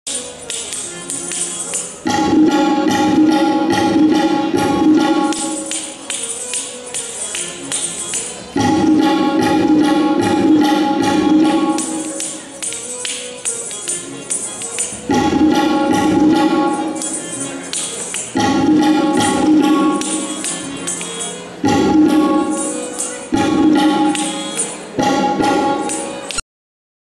Bhakti Singhwane dance competition
Bhakti Singhwane participated in 10′th All India Dance and Drama Competition, recently organised by Gurgaon-based Nistha Sanskritik Manch (NSM). She participated first time in national level Dance Competition and got the 1′st Prize in solo dance competition and 2nd Prize in group dance. Her age is only 8 years.